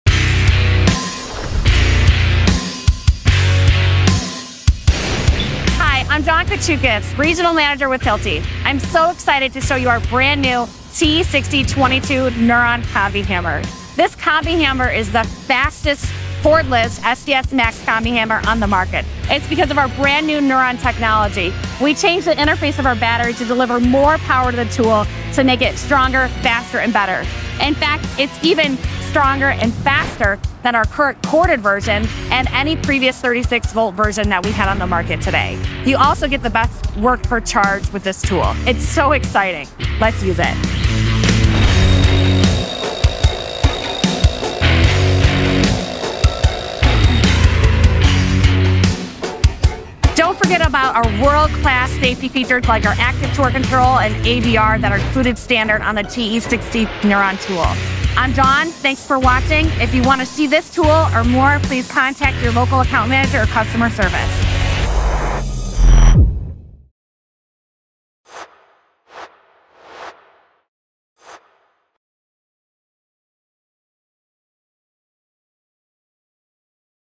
Demonstration of Hilti Nuron TE 60-22 Cordless SDS Max Combihammer for concrete drilling
In this video, Daun Kouchoukos with Hilti demonstrates the new Hilti Nuron TE 60-22 cordless rotary hammer SDS Max with built-in safety features for heavy-duty drilling and chiseling in concrete. Hilti 22 Volt Nuron battery platform.